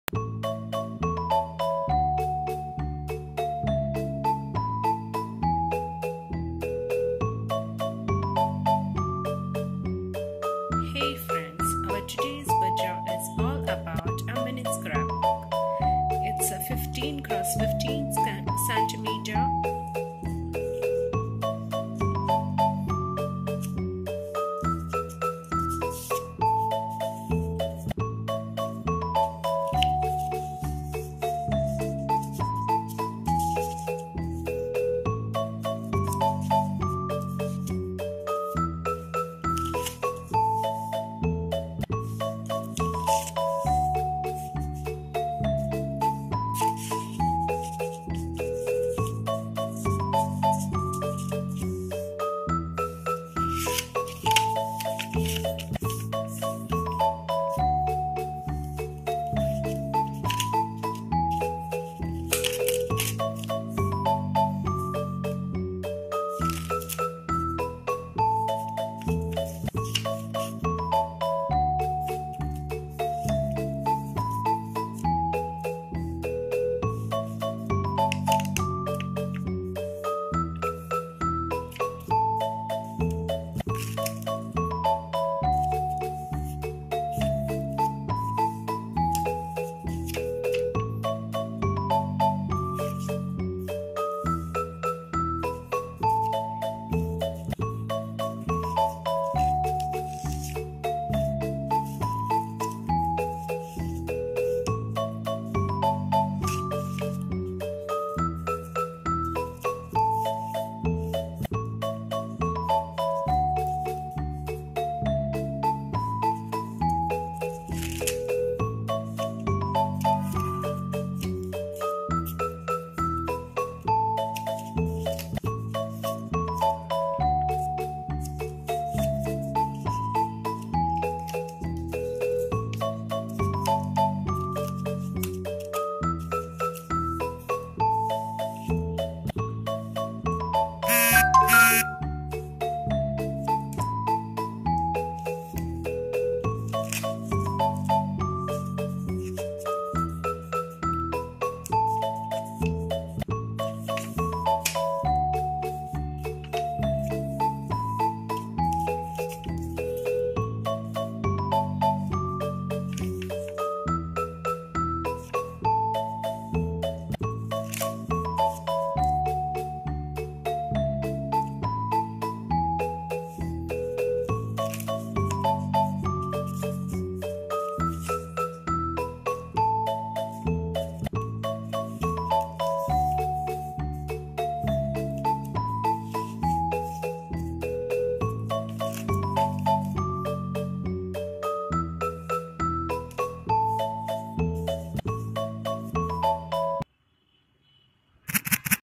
DIY- Mini scrapbook ideas// How to make mini scrapbook for birthday /anniversary
Hello friends!

Today's video is about Mini scrapbook.

How u can make mini scrapbook for gifting in  birthday or anniversary.

How to make different  cards for scrapbook.

My upcoming videos will be of how to make that cards.

Do comment for any help n details.